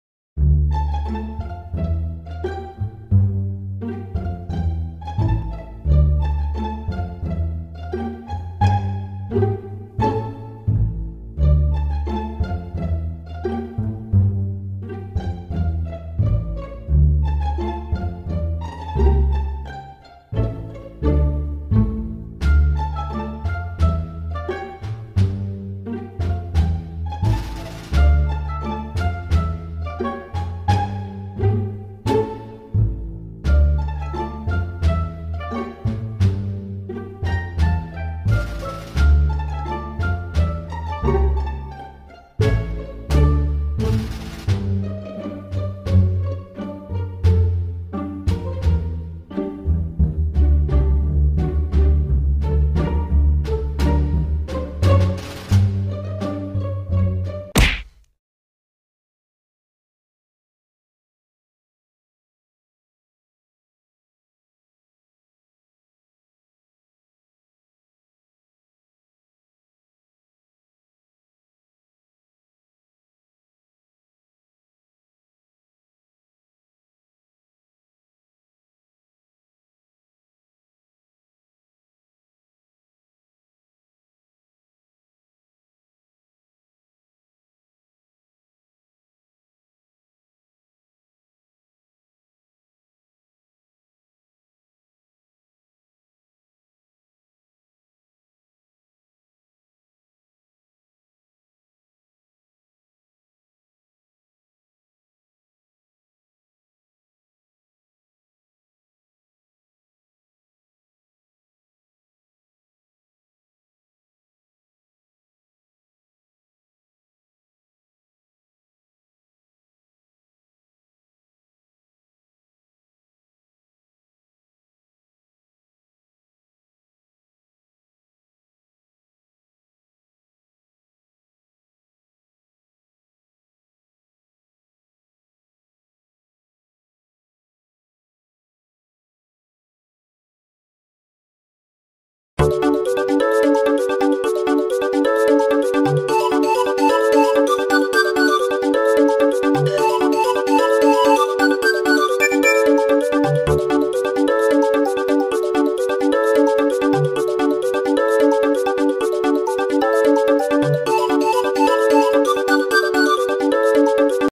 Jenny Wakeman Xj9 Gave Mr.Rude A Nosebleed On His 18Th Birthday
NOTE: this is not grounded videos and ungrounded videos its just the original content